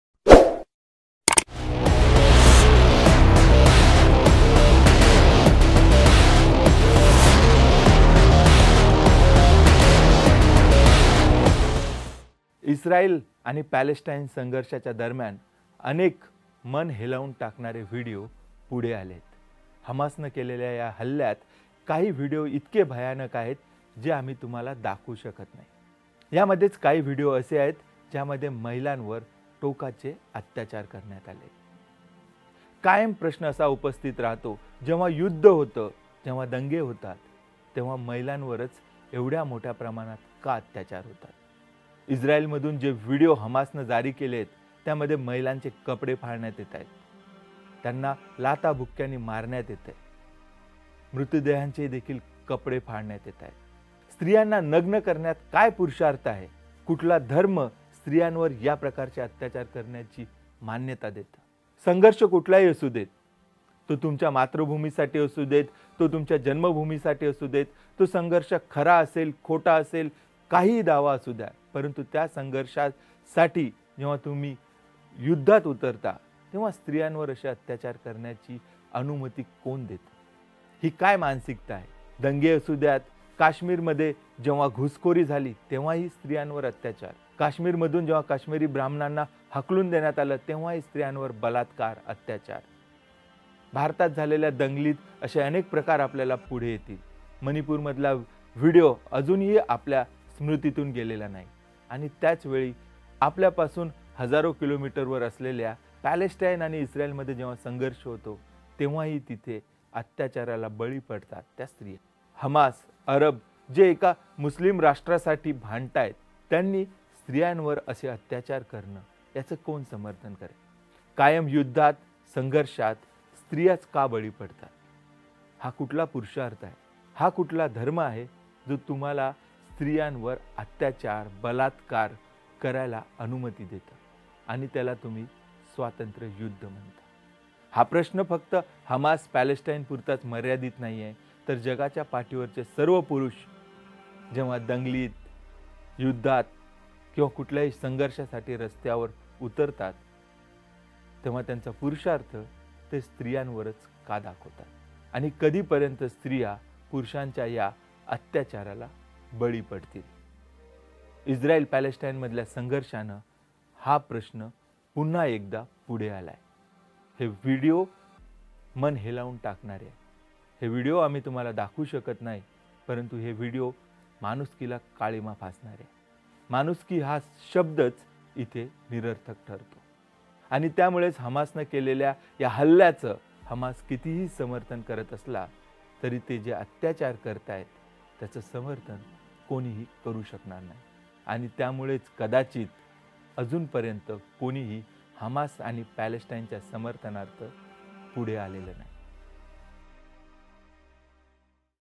Hamaas Terrorist : हमासच्या दहशतवाद्यांचं हैवानी कृत्य... | N18V
हमासच्या दहशतवाद्यांचं हैवानी कृत्य. मृत महिलांचीही करतायत विटंबना. इस्रायली महिलाची काढली नग्न धींड. युद्धात , दंग्यात कायम महिलांवरच का होतात अत्याचार. हा कसला पुरुषार्थ आणि हे कुठलं  युद्ध?

N18V